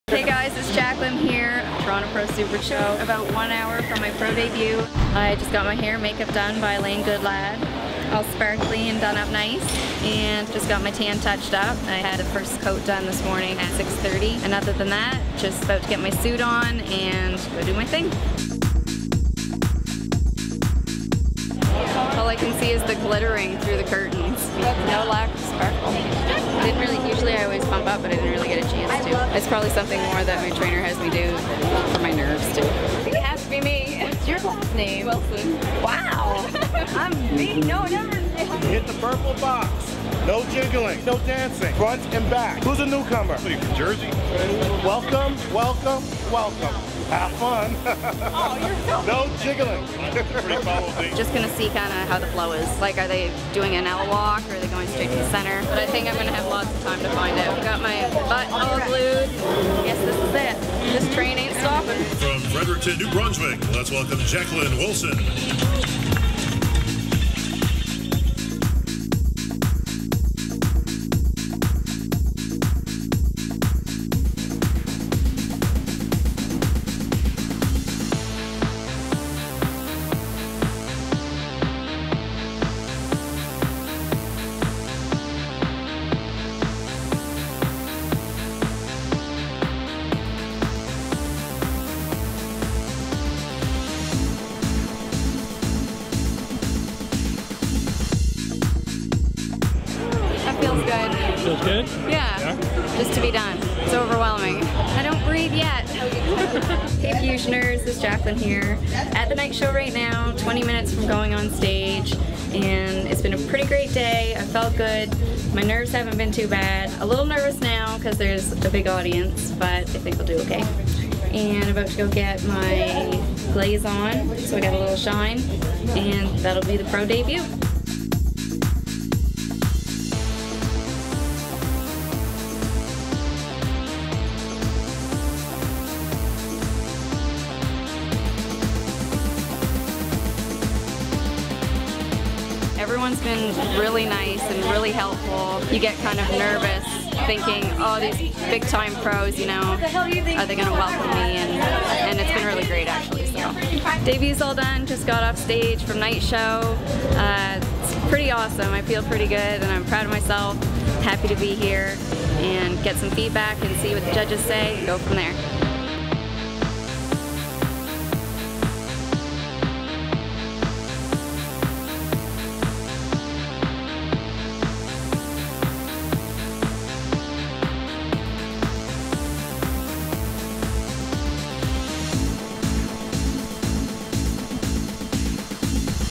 BEING A BODYBUILDER #31: JACLYN WILSON AT THE TORONTO PRO SUPERSHOW 2012
BEING A BODYBUILDER #31 • JACLYN WILSON'S PRO DEBUT - TORONTO PRO SUPERSHOW 2012

Do you remember the first time you stepped onstage? Now think about stepping onstage at a top-level IFBB pro show for the first time. In this episode, we follow Jaclyn Wilson as she does just that.